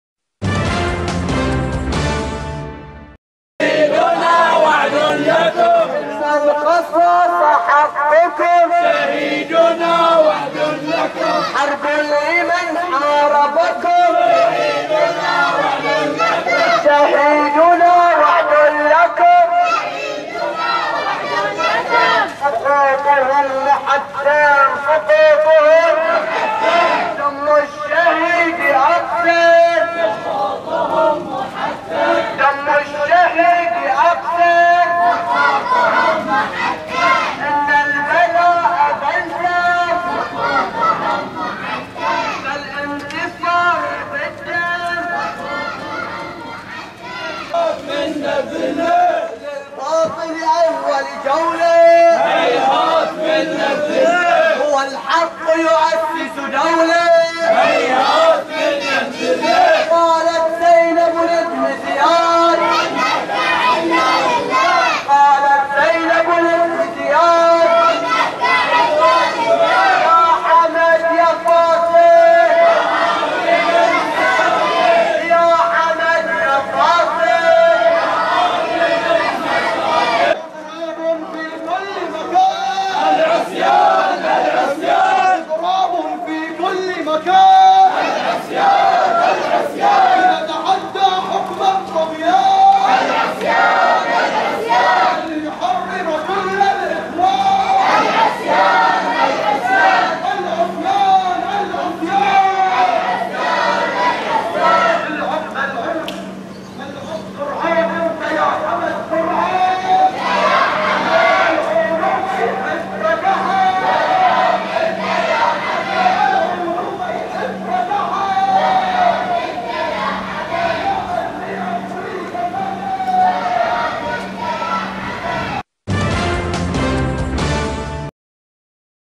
النويدرات : مسيرات متواصلة للمطالبة بإسقاط النظام 16/8/2013 | Bahrain
تواصلت التظاهرات المنادية بإسقاط النظام الملكي الحاكم في البحرين ضمن فعاليات "عاصفة التمرد" والتي تستمر لليوم الثالث على التوالي، ظهر اليوم الجمعة (16 اغسطس/آب 2013) شارك حشد من المواطنين في مسيرة خرجت بمنطقة النويدرات، حمل المشاركون فيها أعلام البحرين ورددوا شعارات تطالب بإسقاط حاكم البحرين حمد بن عيسى آل خليفة.